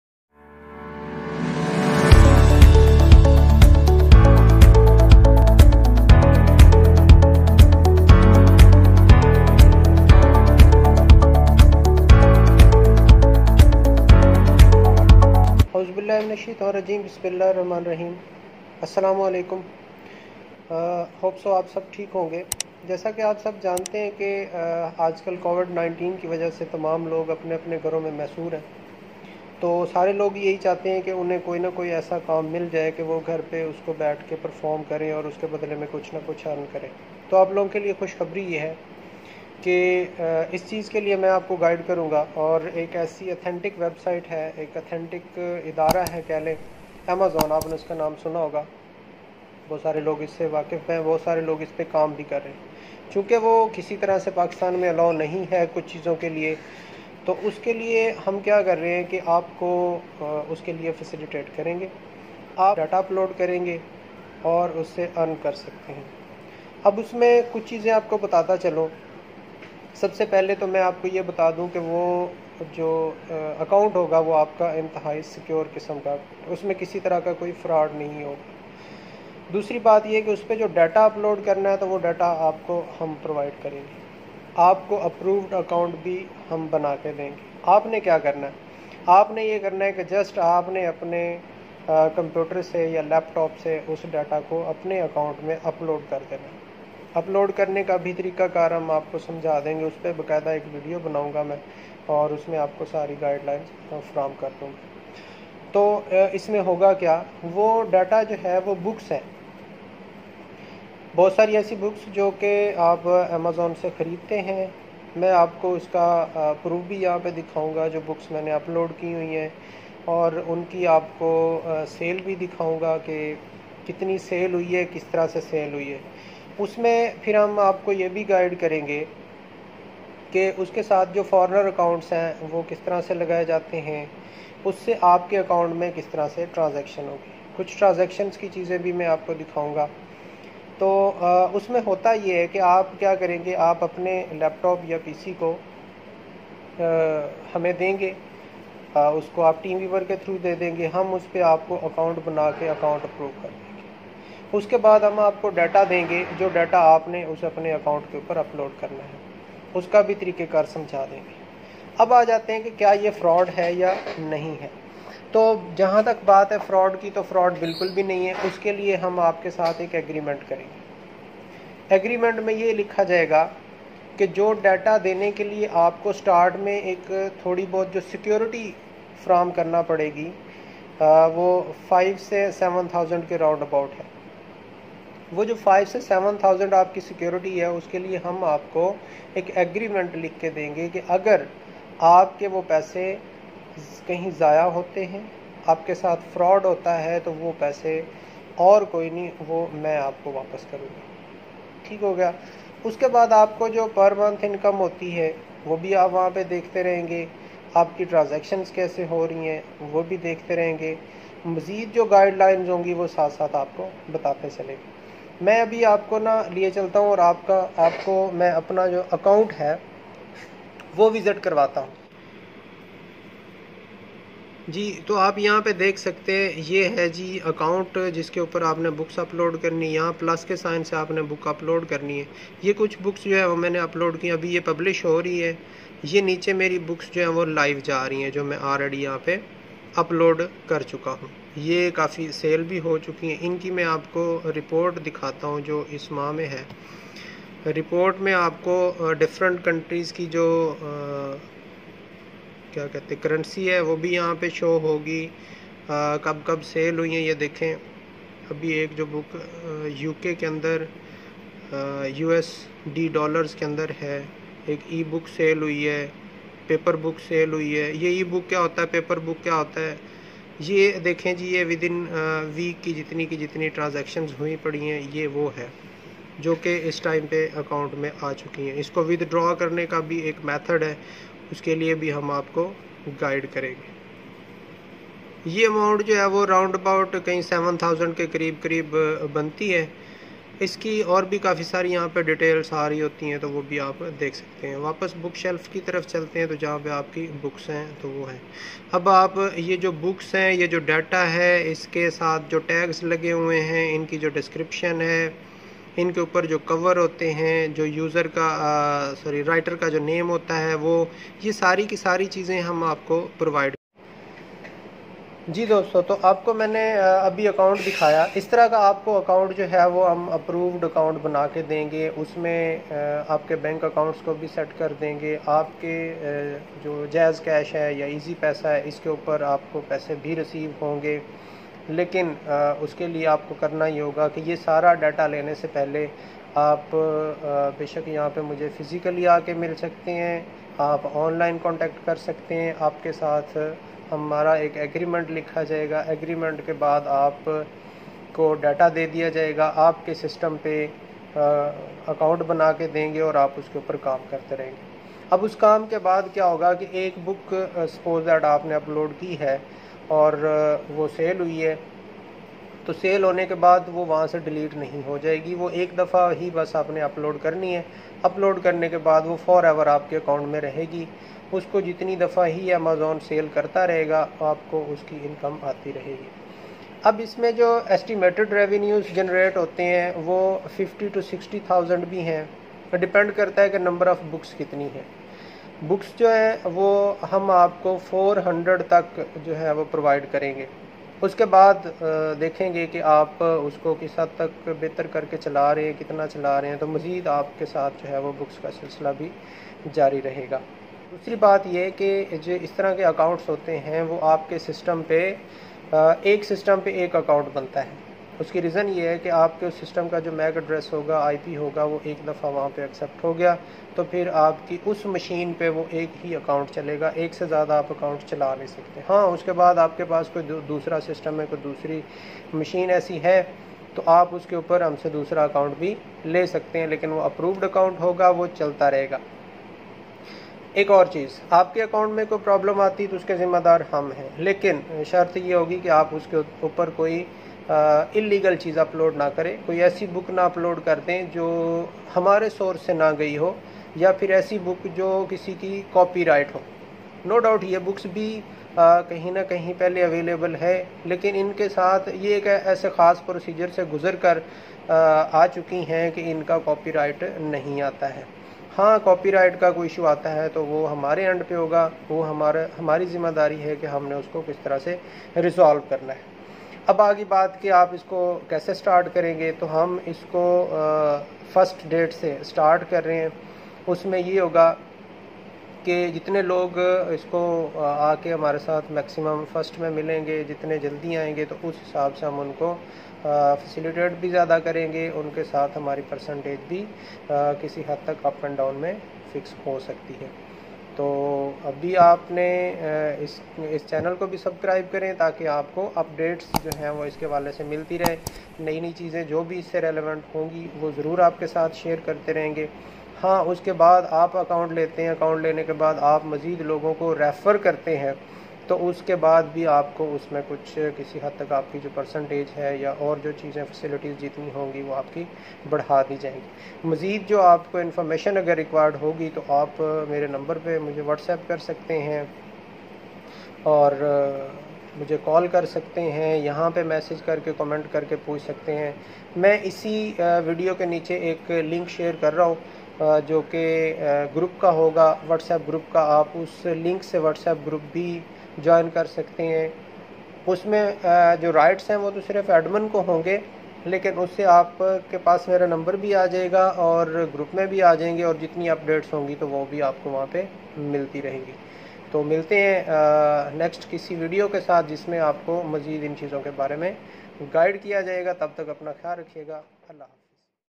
How to earn online with amazon KDP|| Online copy past || KDP books online
In this video i am going to show you how to earn from amazon with approved account just copy past data and earn 300& per month just do it one time and earn whole life
Whatapp Group link for updates